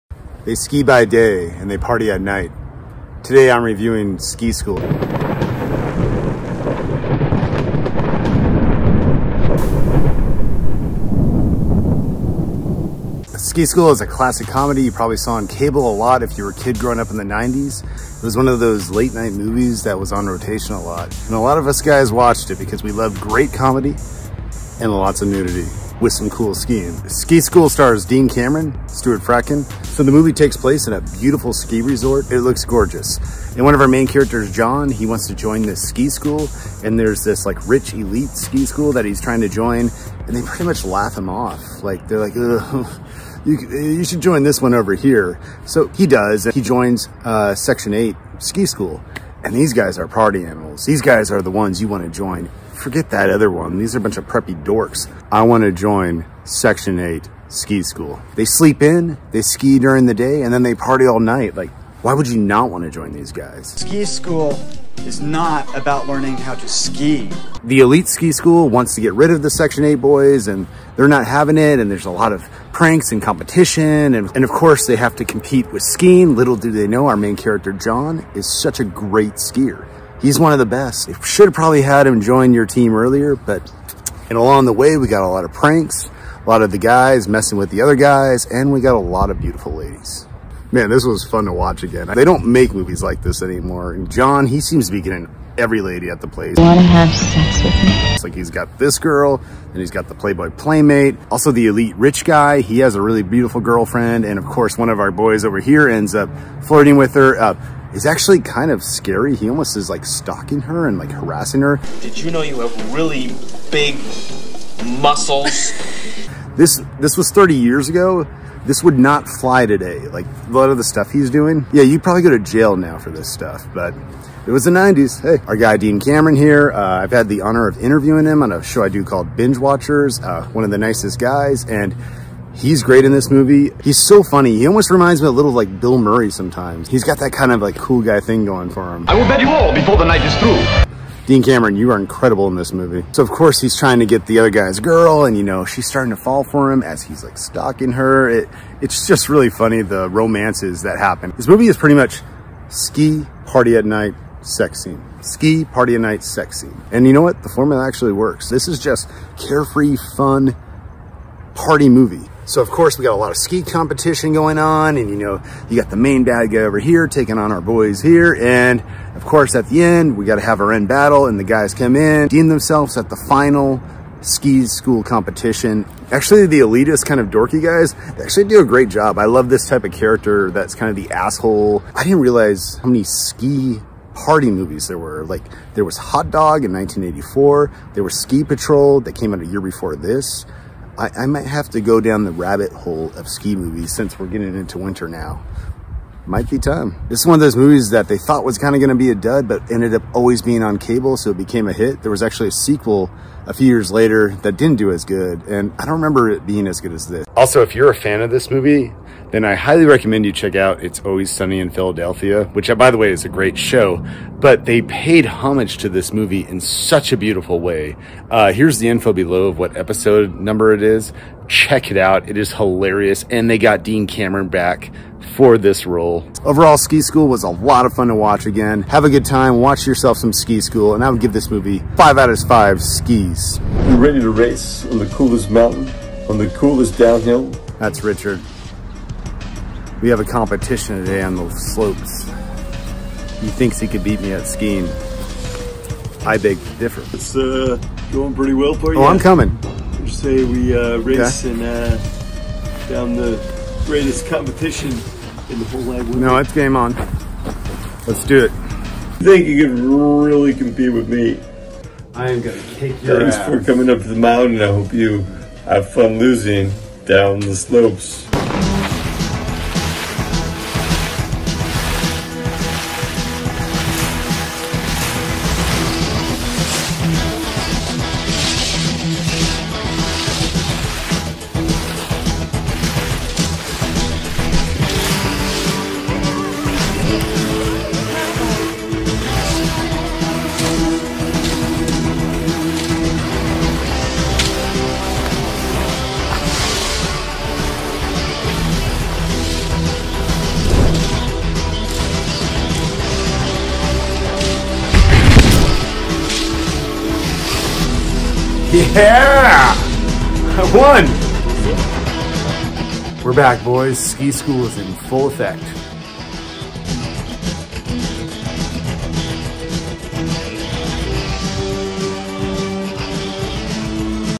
"Ski School" Movie Review
Dave Marshak (Dean Cameron) and his friends are a pack of highly skilled skiers and devoted party animals. On the slopes, they are known as "Section 8." But their reputation is at risk when a ski school of snobs -- headed by pompous and preppy Reid Janssens (Mark Thomas Miller) -- shows up looking t… More
Initial release: January 11, 1991
Director: Damian Lee

Starring:
Jean Dean
Shane Jesperson

Edited By:
Nick Thiel